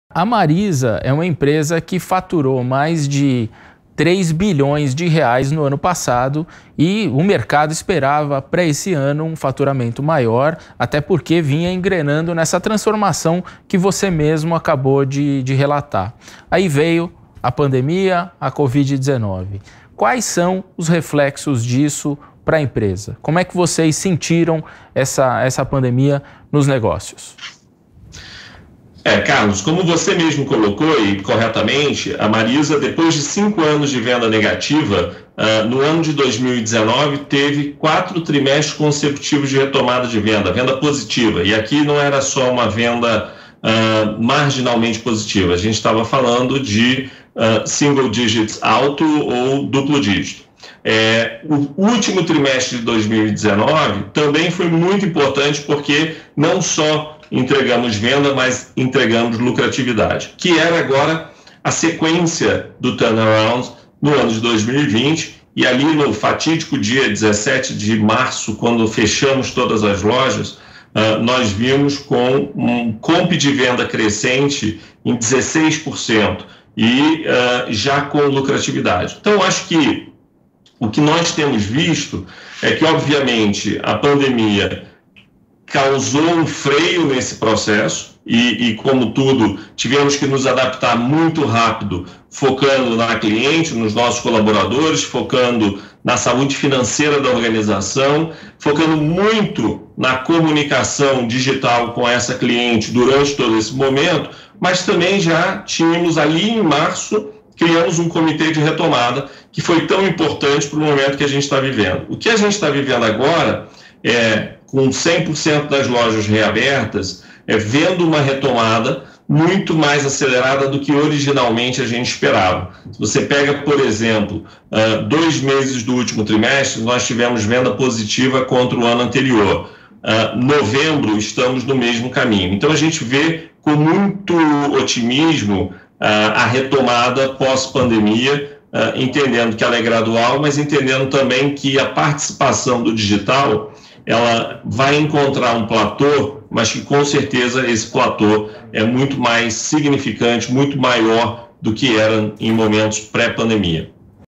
Como a pandemia afetou a lucratividade dos negócios | Conselho de CEO
Marcelo Pimentel conta como a Marisa lidou com a pandemia, que acabou freando o momento de retomada dos negócios, e destaca o foco na comunicação digital com os clientes como expoente dos lucros.